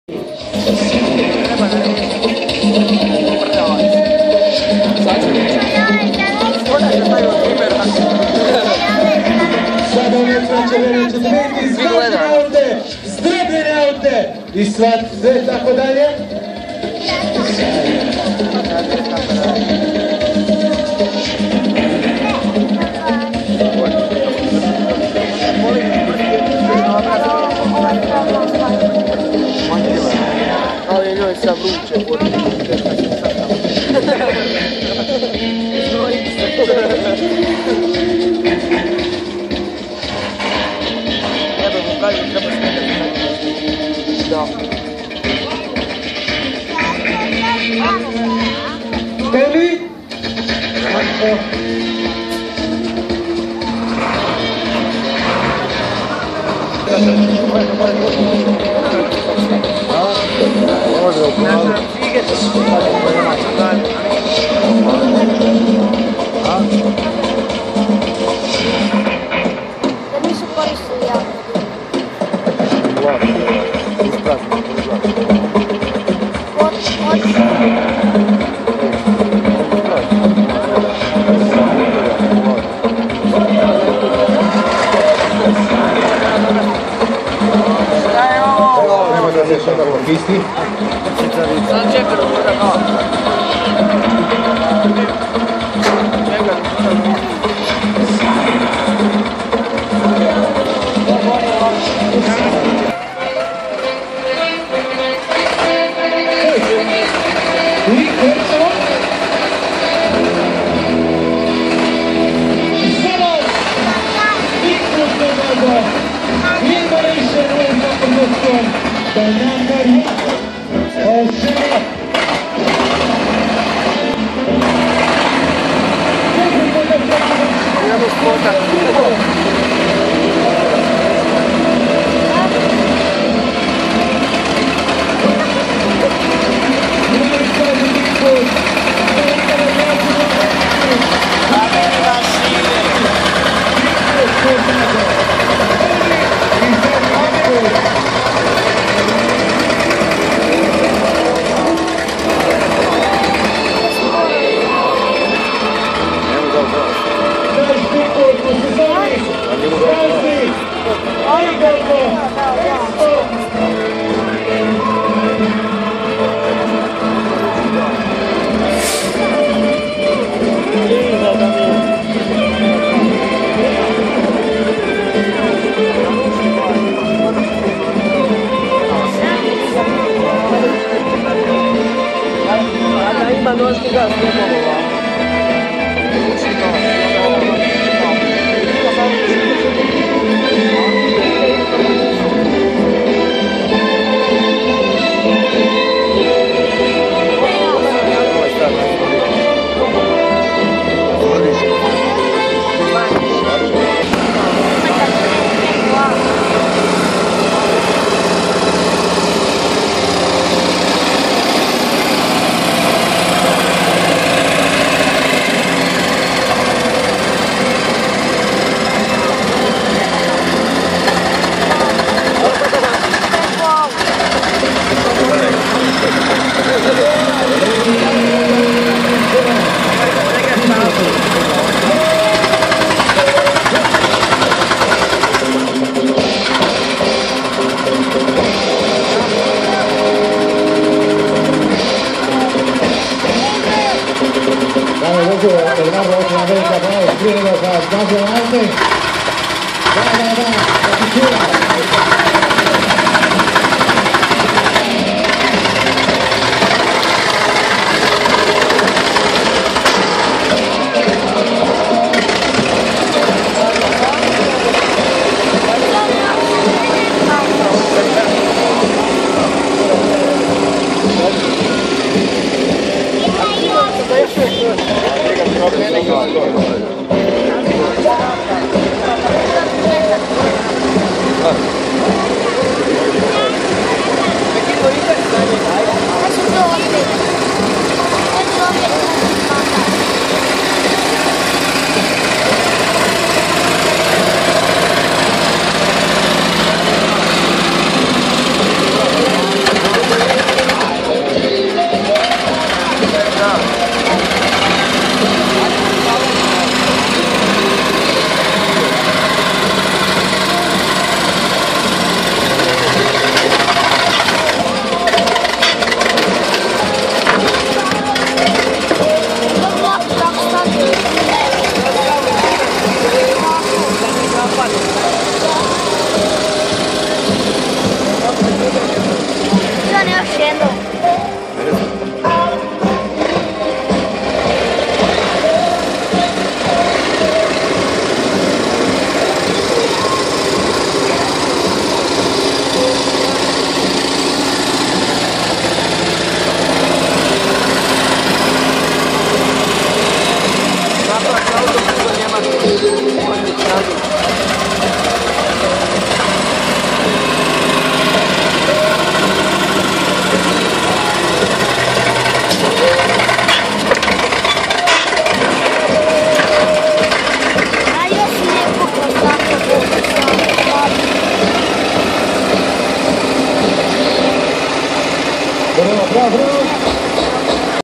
Stunt show Slavonski Brod 2015-Monster truck drive
Slavonski Brod (Croatia), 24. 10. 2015.
Mexican stunter named Eduardo came here today to show us some crazy stunts and vehicles mostly driven in America. In this video it's Monster truck.